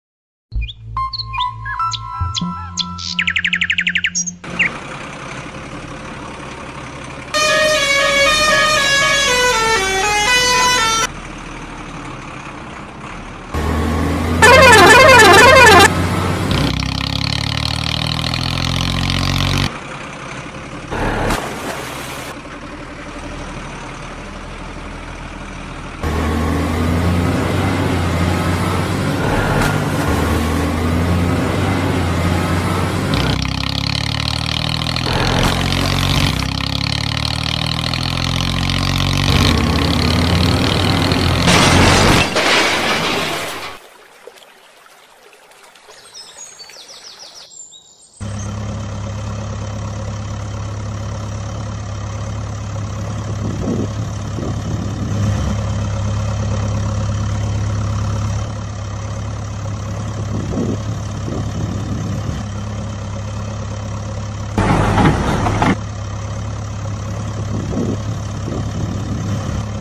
Hywa Truck and Mini Tractor Stucked in deep hole Jcb 5cx pulling out | Apaar Entertainment
Jcb 5cx Pulling out Stucked Auto Riksha From deep pit | Apaar Entertainment |

Title :- Hywa Truck and Mini Tractor Stucked in deep hole Jcb 5cx pulling out | Apaar Entertainment |
Channel Handlers :-  SRK

                            ヾ(＾-＾)ノ